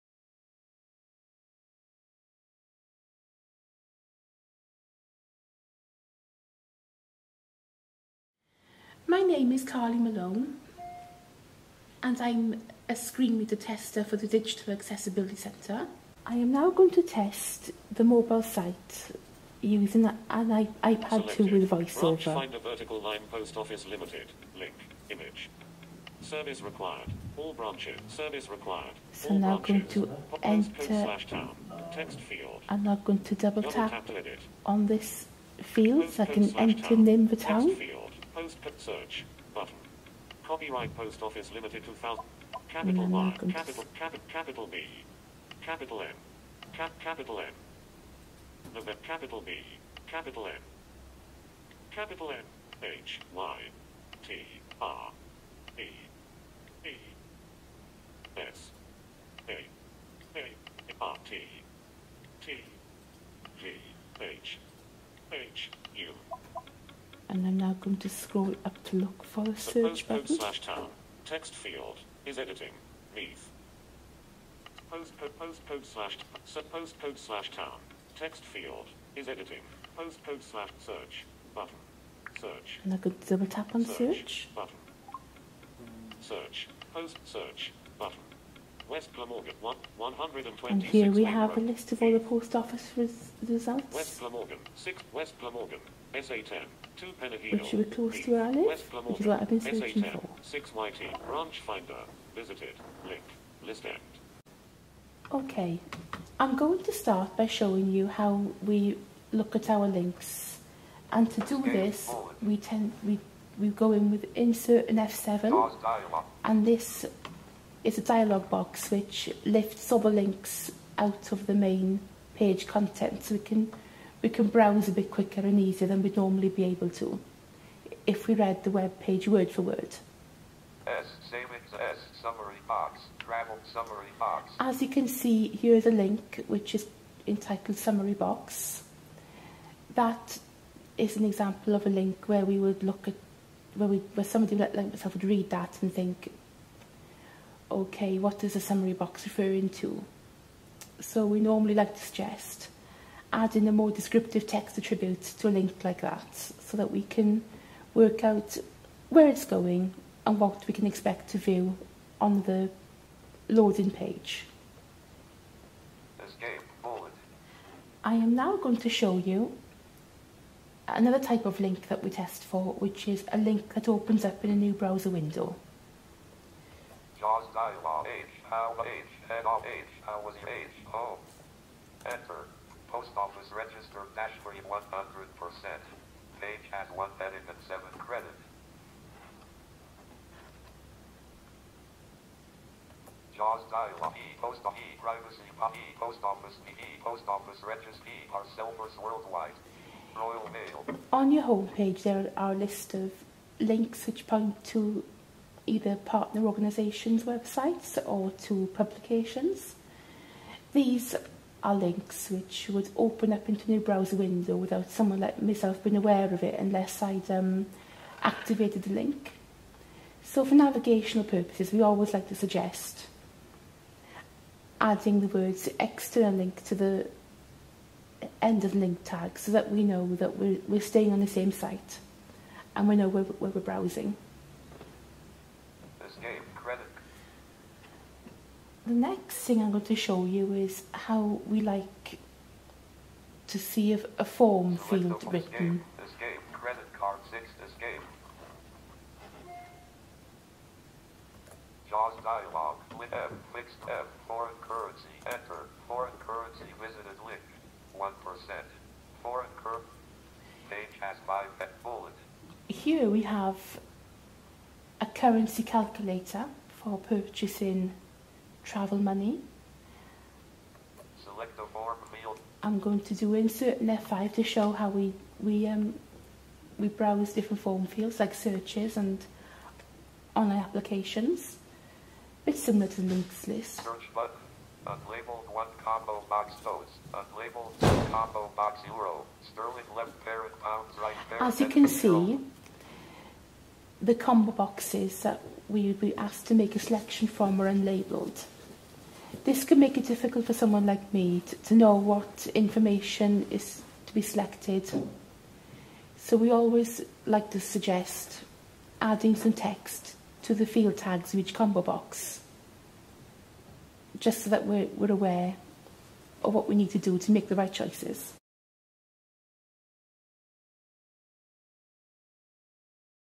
Digital Accessibility Centre - iOS and Desktop Screen Reader Demo - Carly (Post Office)
Carly briefly takes us through a screen reader demo of VoiceOver on iOS and JAWS on desktop